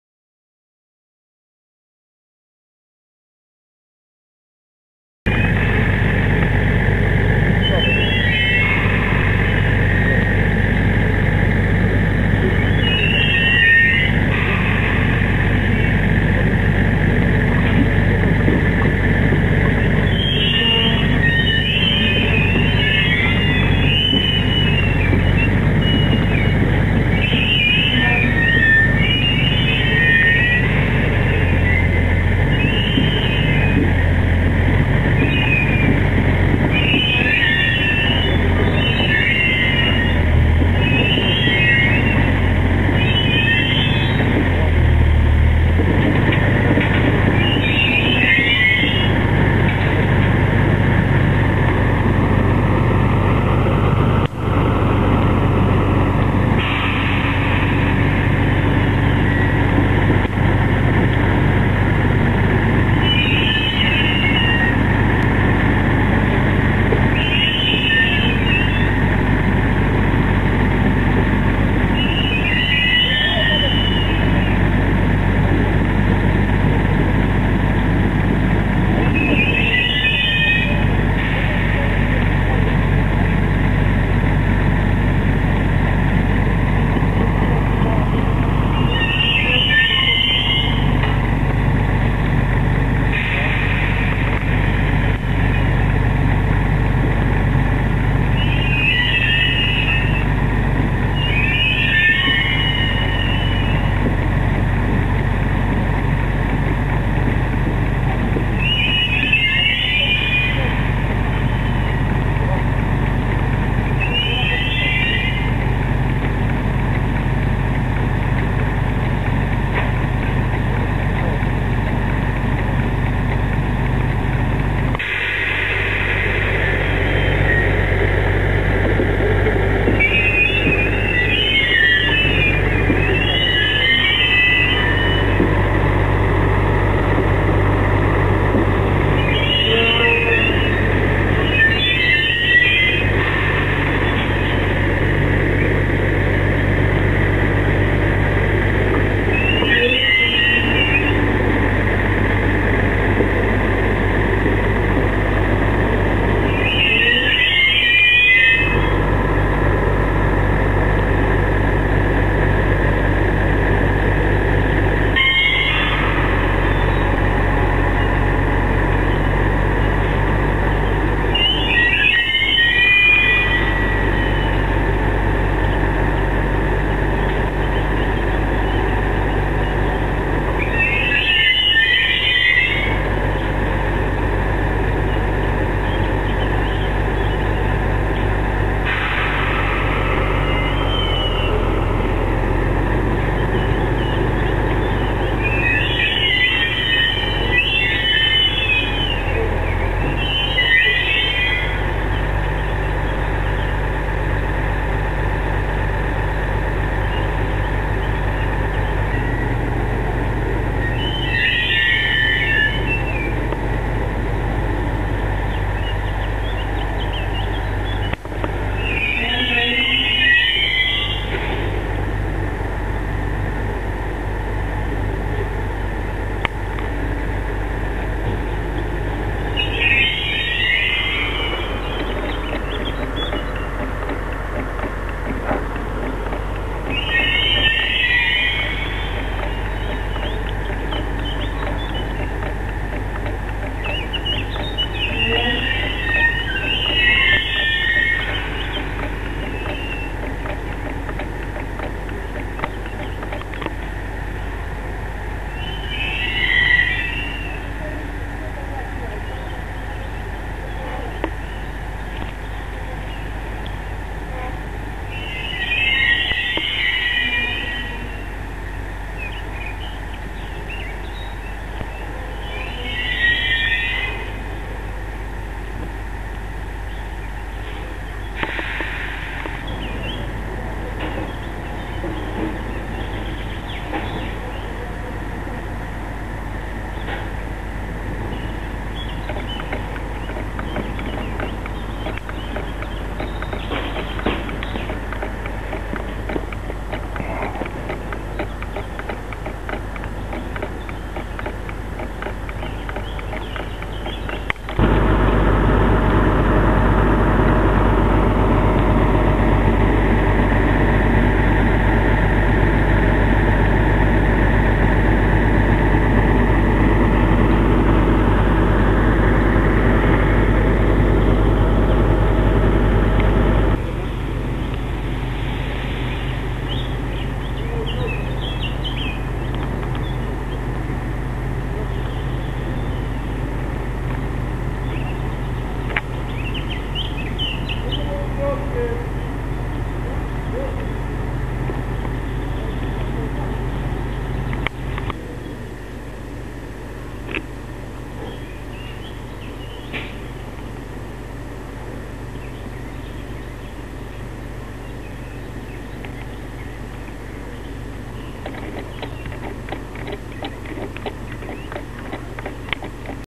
Naprawa sieci trakcyjnej po kradzieży
Naprawa sieci trakcyjnej w Bielowicku na 17 kilometrze na odcinku Pogórze - Grodziec Śląski w dni 1 maja 2009